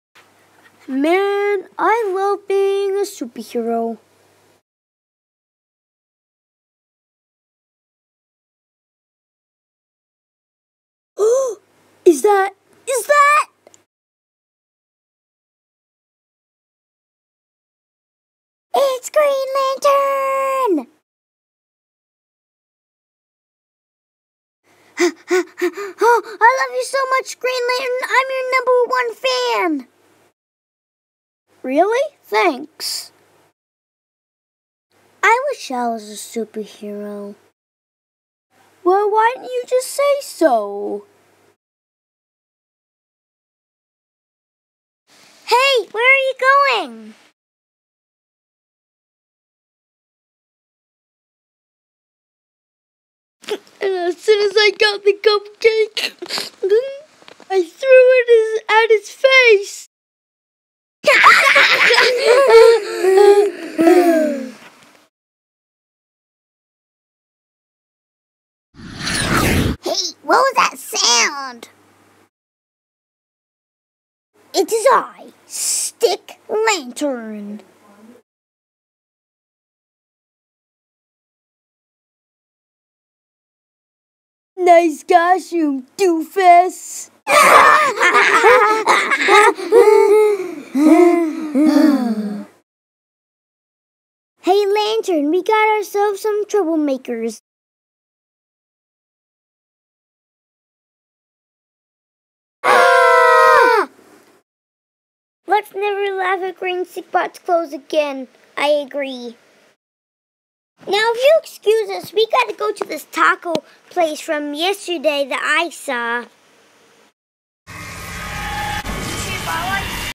stiklantern the movie 2017 HD
this is a video about a stikbot who always wanted to be a super hero but green lantern helps him but is he good enough to stop the mean bad guys... find out the movie stiklanten